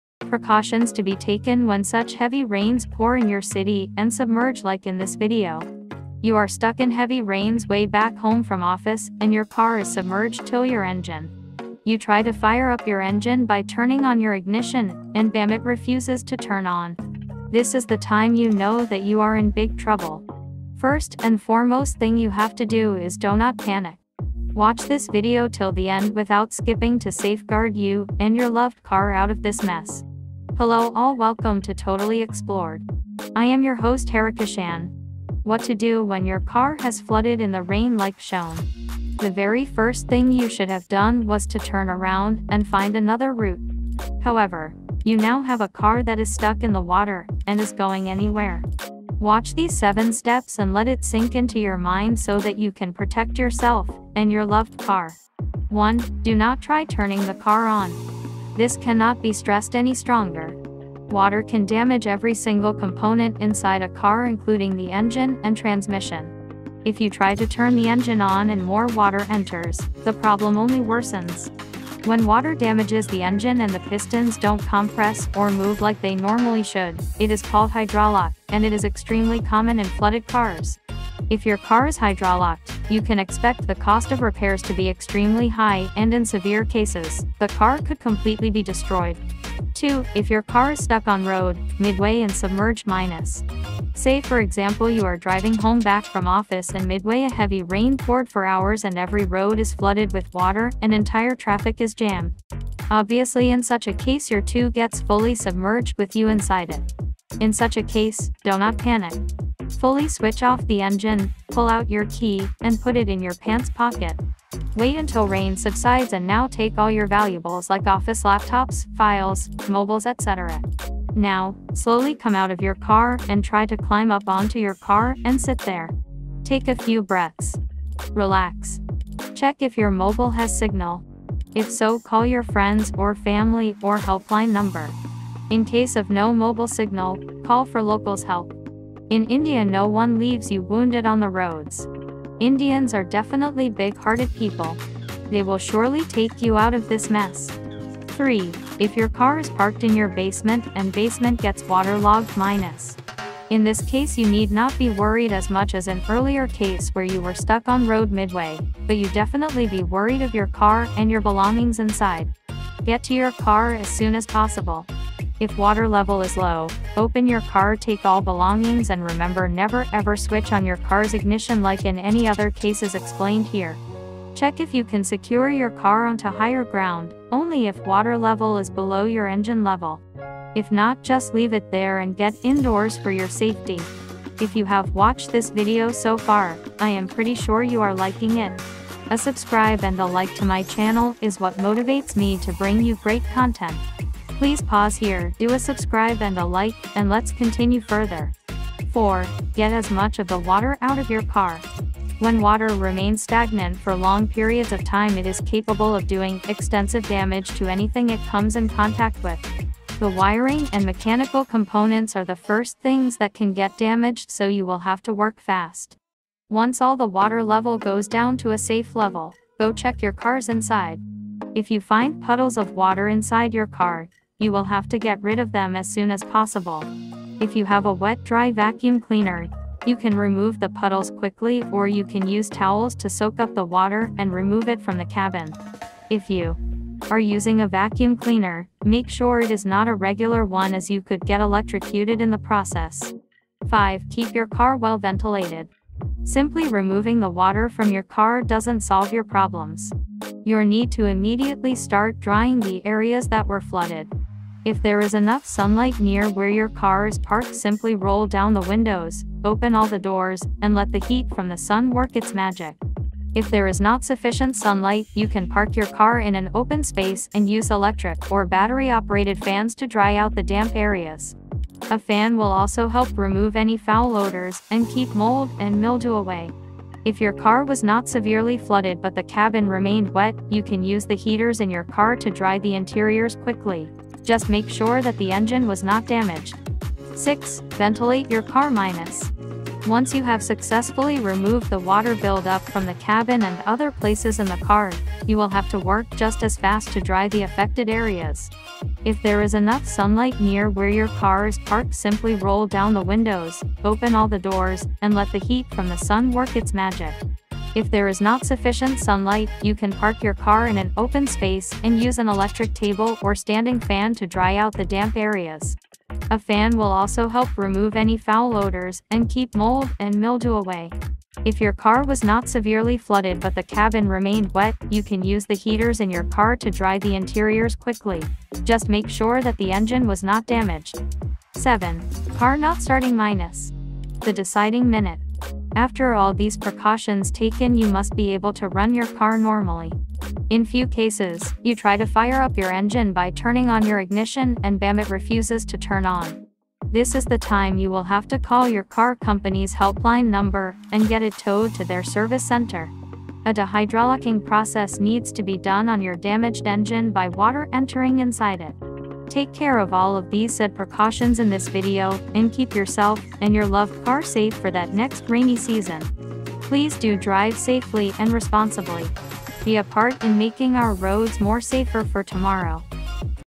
Car Submerged & stuck car in heavy rains? Safety Do's and Don'ts
Save your loved car - Submerged, flooded car - by HariKishan.

precautions to be taken when such heavy rains pour in your city and submerge like in this video:

You are stuck in heavy rains way back home from office and your car is submerged till your engine. You try to fire up your engine by turning on your ignition and bam it refuses to turn on. This is the time you know that you are in big trouble. First and foremost thing you HAVE to do, is DONOT PANIC!!! Watch this video till the end without skipping to safeguard you and your loved car out of this mess.

Hello all, welcome to Totally Explored. I am your host HariKishan.

What to Do When Your Car Has Flooded in the Rain like shown.
The very first thing you SHOULD HAVE DONE was to turn around and find another route. However, you now have a car that is stuck in the water and is going anywhere. Watch these 7 steps and let it sink into your mind so that you can PROTECT yourself and your loved car.

Take care of all of these said precautions in this video, and keep yourself and your loved car safe for that next rainy season.
Please do drive Safely and responsibly. Be a part in Making our roads more safer for tomorrow.